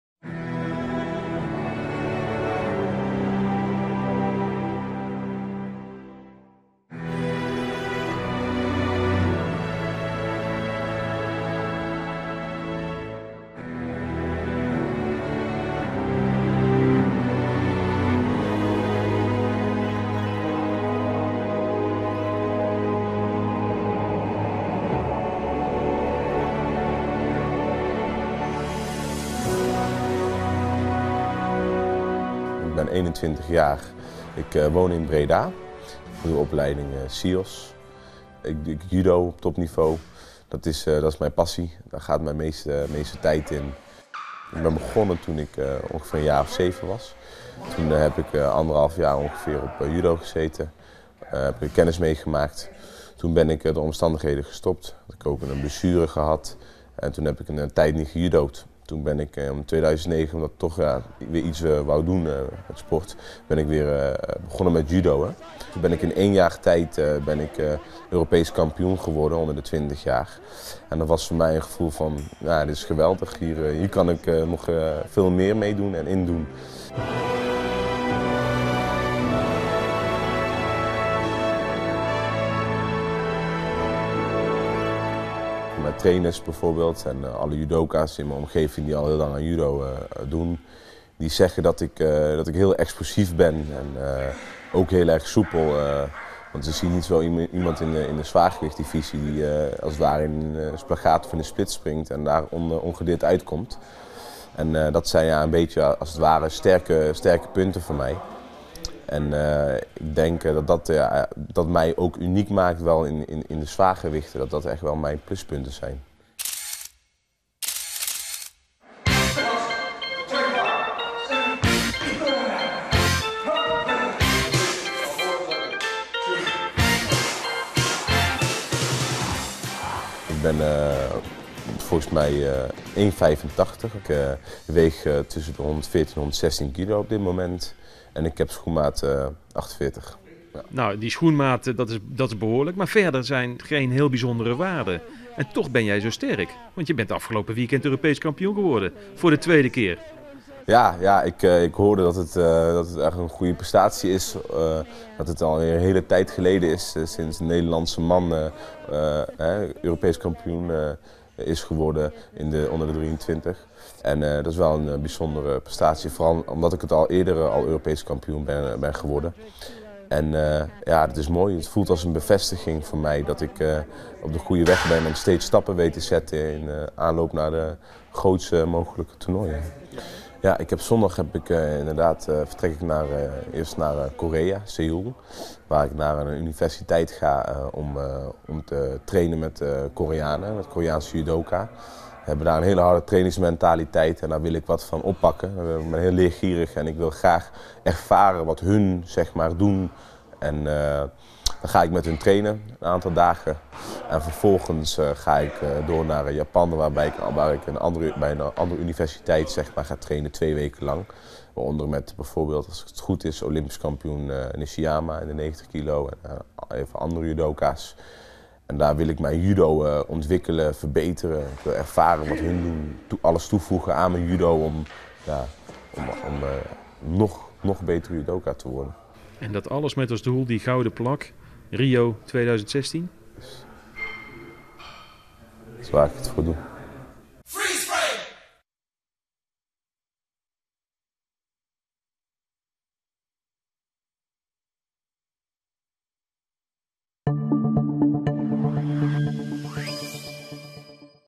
Judokampioen Roy Meyer naar Azië
De kersverse Europees kampioen judo Roy Meyer vertrekt dit weekend naar Zuid-Korea en Japan voor een trainingskamp. De 21-jarige Bredanaar dankt deze uitnodiging aan de Europese titel die hij afgelopen weekend won in Tsjechië.